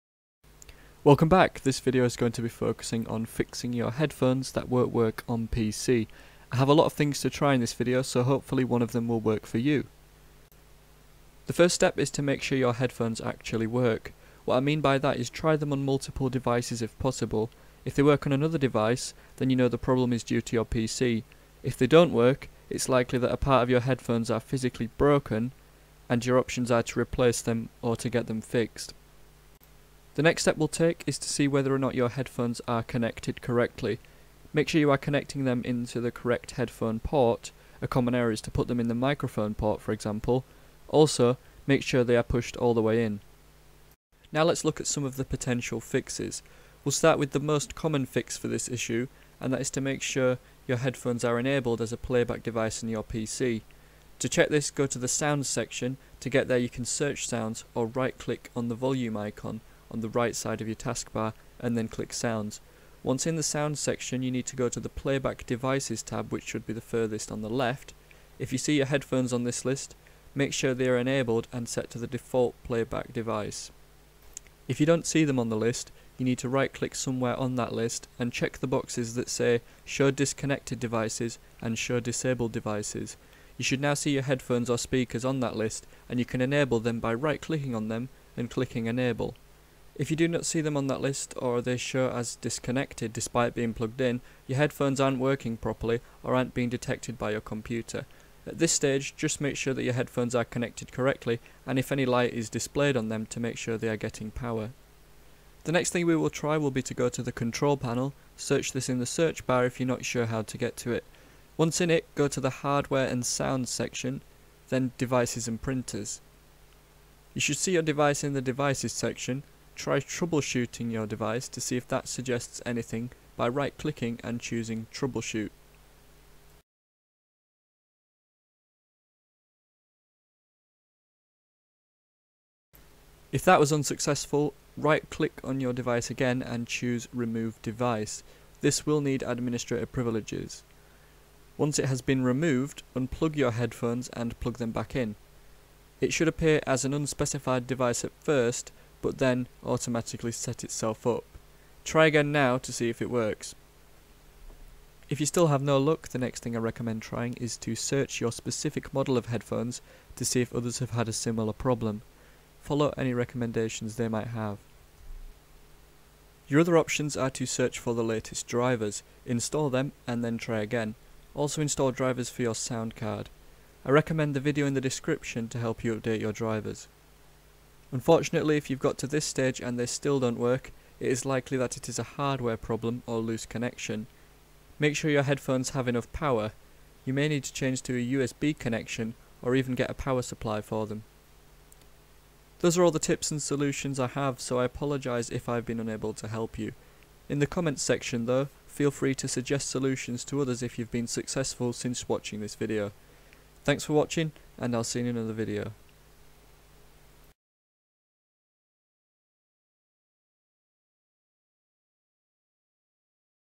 Headphones Don't Work On PC? - How To FIX Headphones Not Working on Windows 10
Headphones not working on PC/Windows 10 or stopped working? Your headphones may also be not detected in Windows 10. This video will show you how to make your headphones work if headphones don't work on PC. I have numerous ways to fix the problem so hopefully one will work! The methods should also help fix headphones not working on the upcoming Windows 11.

Headphones may not be working on Windows 10 for a number of reasons, it could be to do with your headphones not being enabled as a playbacks device, outdated drivers, not enough power or a dodgy connection. This video should help with most of the common issues that stop headphones working.